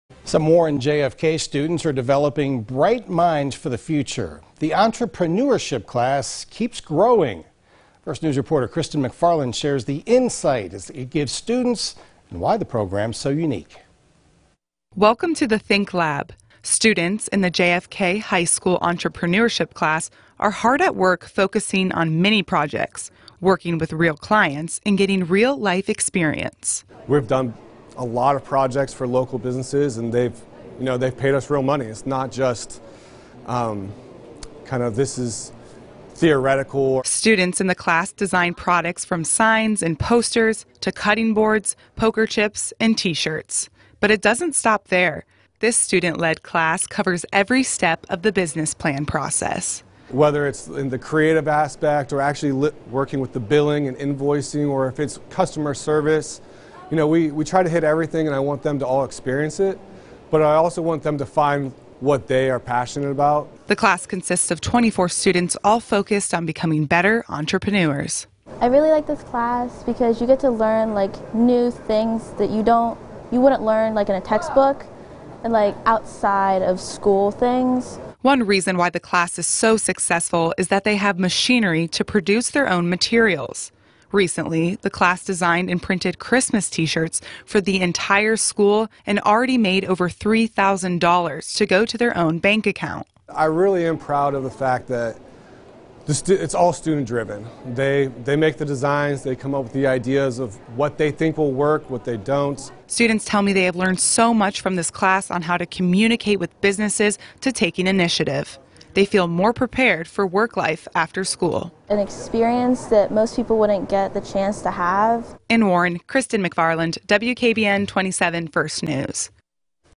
JFK High School class gives students springboard to career goals
In the "Think Lab," students are hard at work focusing on many projects, working with real clients and getting real-life experience.